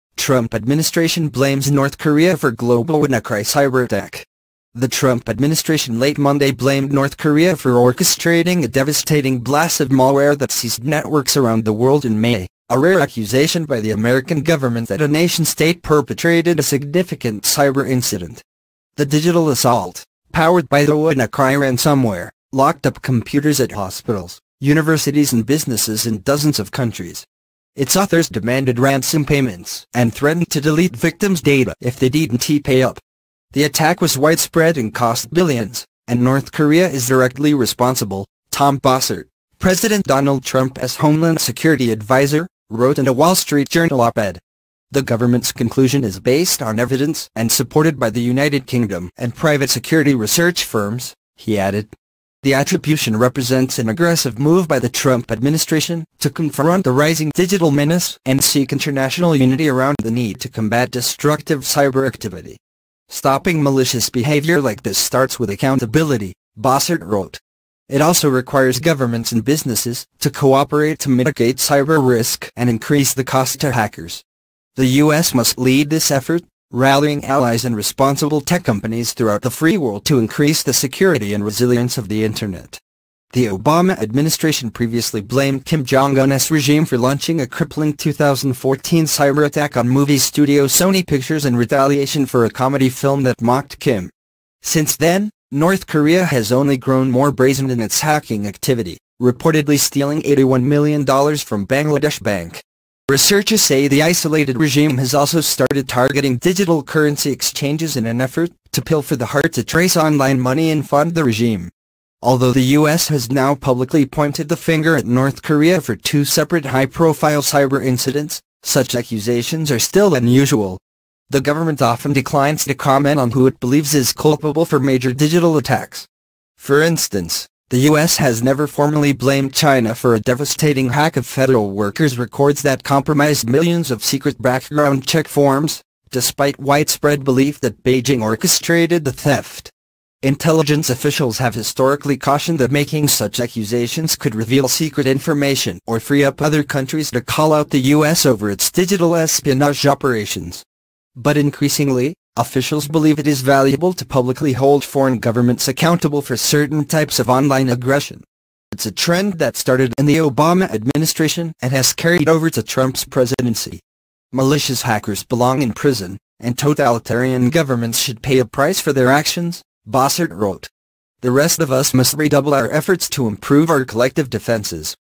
Trump administration blames North Korea for global WannaCry cyberattack
The Trump administration late Monday blamed North Korea for orchestrating a devastating blast of malware that seized networks around the world in May, a rare accusation by the American government that a nation-state perpetrated a significant cyber incident.

The digital assault, powered by the WannaCry ransomware, locked up computers at hospitals, universities and businesses in dozens of countries. Its authors demanded ransom payments and threatened to delete victims’ data if they didn’t pay up.
The attack was widespread and cost billions, and North Korea is directly responsible,” Tom Bossert, President Donald Trump’s homeland security adviser, wrote in a Wall Street Journal op-ed.

The government's conclusion is “based on evidence” and supported by the United Kingdom and private security research firms, he added.

The attribution represents an aggressive move by the Trump administration to confront a rising digital menace and seek international unity around the need to combat destructive cyberactivity.
“Stopping malicious behavior like this starts with accountability,” Bossert wrote. “It also requires governments and businesses to cooperate to mitigate cyber risk and increase the cost to hackers. The U.S. must lead this effort, rallying allies and responsible tech companies throughout the free world to increase the security and resilience of the internet.”

The Obama administration previously blamed Kim Jong Un’s regime for launching a crippling 2014 cyberattack on movie studio Sony Pictures in retaliation for a comedy film that mocked Kim.

Since then, North Korea has only grown more brazen in its hacking activity, reportedly stealing $81 million from Bangladesh Bank.

Researchers say the isolated regime has also started targeting digital currency exchanges in an effort to pilfer the hard-to-trace online money and fund the regime.
Although the U.S. has now publicly pointed the finger at North Korea for two separate high-profile cyber incidents, such accusations are still unusual.

The government often declines to comment on who it believes is culpable for major digital attacks. For instance, the U.S. has never formally blamed China for a devastating hack of federal workers' records that compromised millions of secret background check forms, despite widespread belief that Beijing orchestrated the theft.

Intelligence officials have historically cautioned that making such accusations could reveal secret information or free up other countries to call out the U.S. over its digital espionage operations.

But increasingly, officials believe it is valuable to publicly hold foreign governments accountable for certain types of online aggression. It's a trend that started in the Obama administration and has carried over to Trump's presidency.

“Malicious hackers belong in prison, and totalitarian governments should pay a price for their actions,” Bossert wrote. “The rest of us must redouble our efforts to improve our collective defenses.”